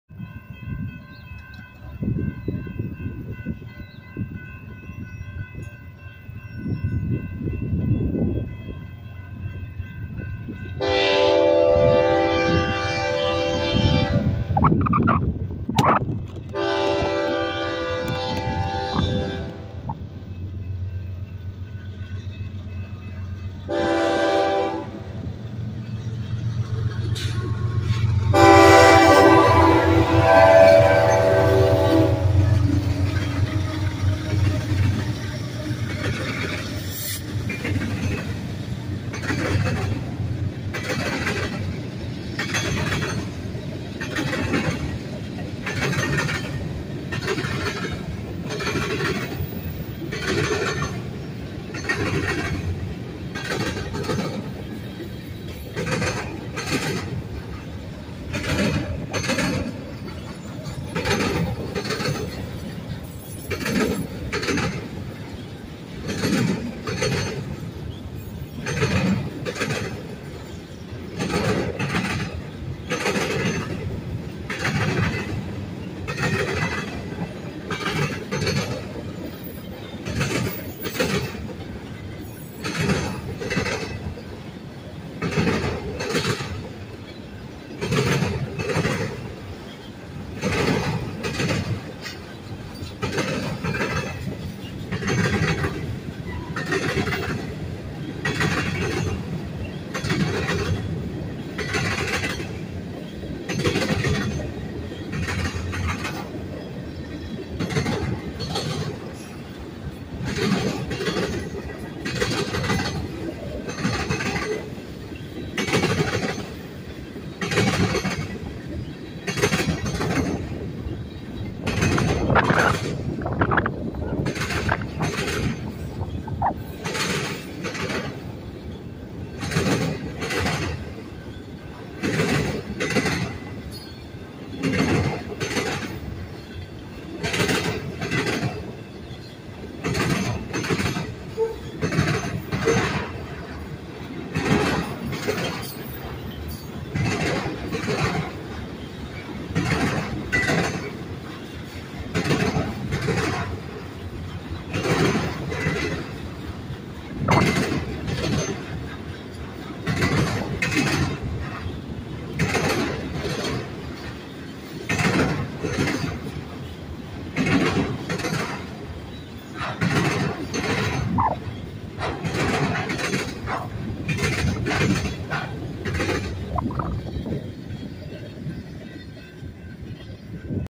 Nb Union Pacific train with KCS&UP power leading. Austin,Tx. 06/10/2023
Kansas City Southern#4786&Union Pacific#6918
leads the Nb train over the Austin Western diamond at McNeil Jct on the Austin Sub.
About 00:16 into the video,the wind blew the tripod over and saved it in time to get the train video.
Austin,Tx.  06/10/2023